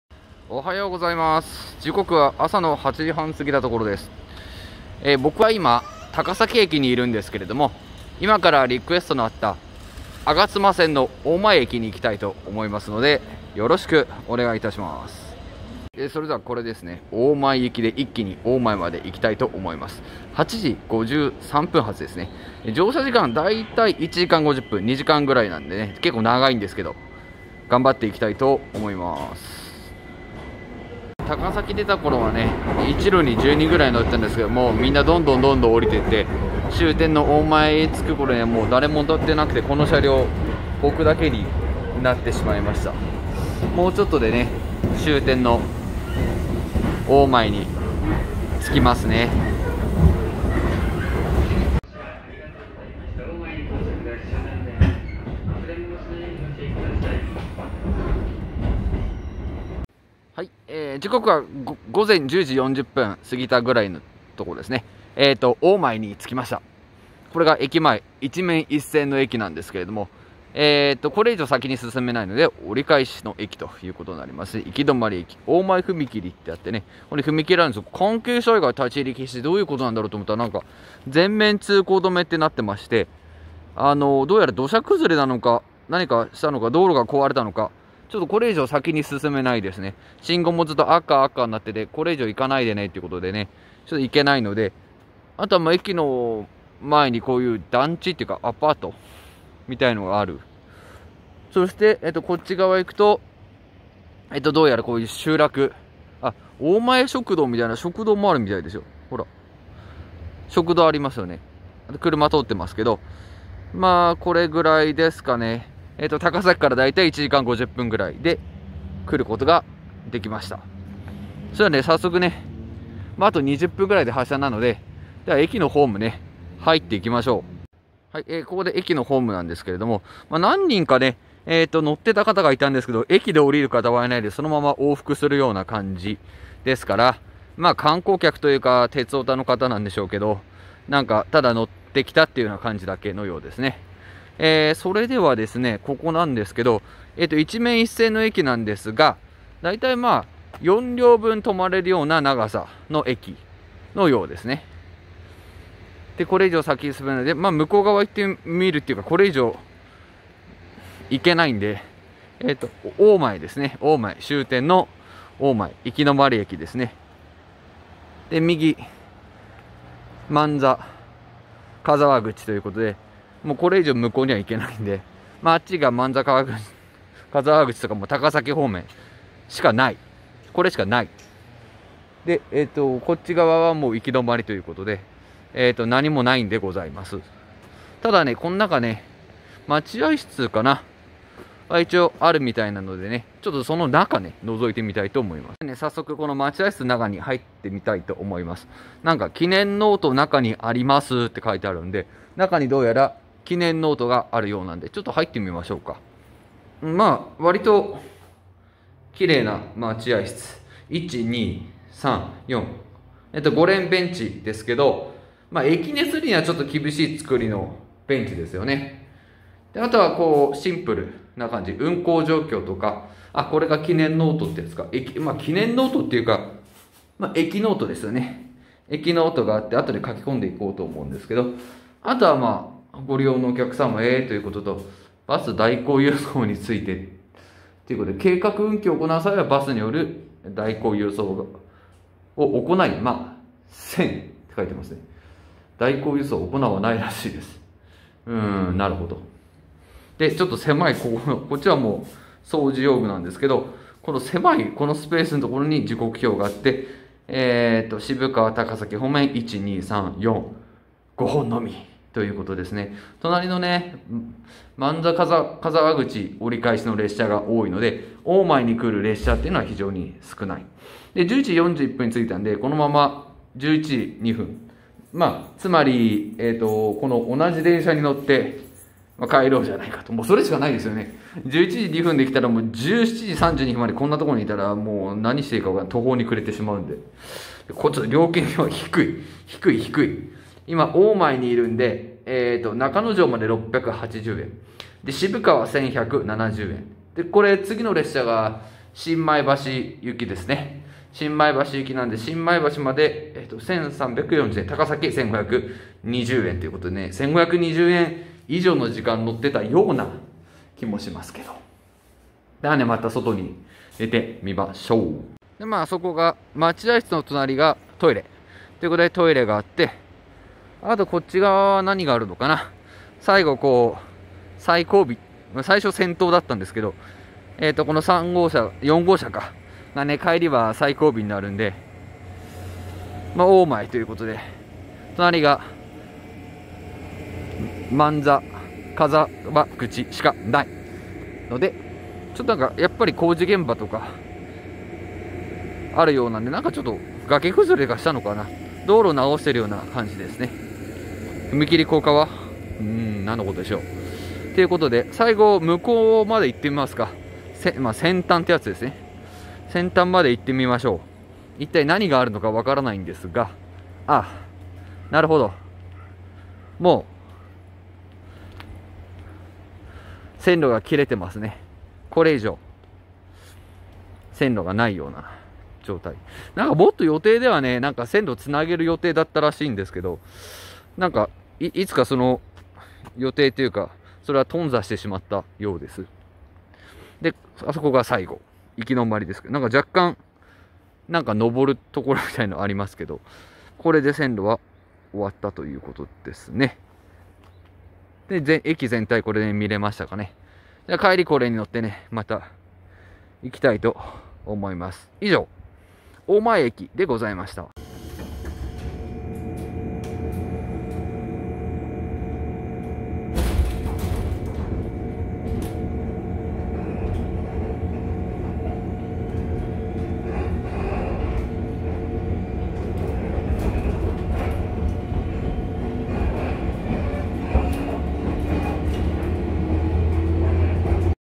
終着駅シリーズ（４）大前駅（吾妻線）
吾妻線終着駅の大前駅です。駅の周りにはアパートと数件の民家がありました。降雨による土砂崩れでしょうか。駅周りは工事中でした。